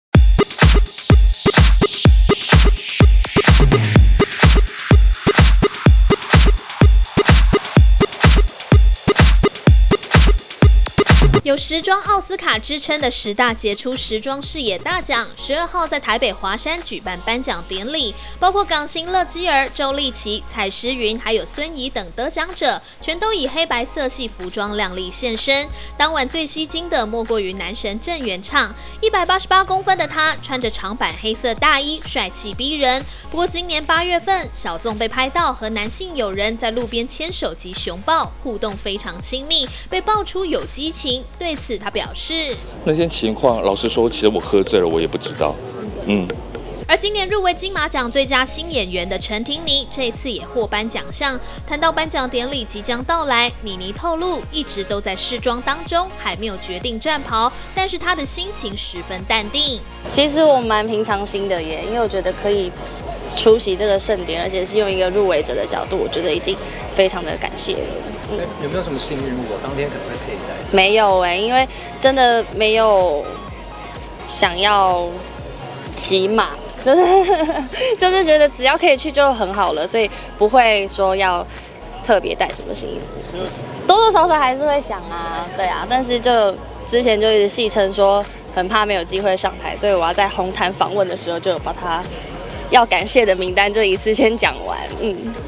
鄭元暢談深夜熊抱磨鼻男友人 坦言喝醉不知自己做啥
有時裝奧斯卡之稱的十大傑出時裝視野大獎，
12日在台北華山舉辦頒獎典禮，
包括港星樂基兒，周麗淇，蔡詩芸還有孫怡等得獎者，
全都以黑白色系服裝亮麗現身。
當晚最吸睛的莫過於男神鄭元暢現身，
188公分的他，穿著長版黑色大衣帥氣逼人。
不過今年8月份，小綜被拍到和男性友人在路邊牽手及熊抱，
互動非常親密，被爆出有基情，對此他表示。

而今年入圍金馬獎最佳新演員的陳庭妮，
這次也獲頒獎項，談到頒獎典禮即將到來，
妮妮透露一直都在試裝中，還沒決定戰袍，
而心情也十分淡定。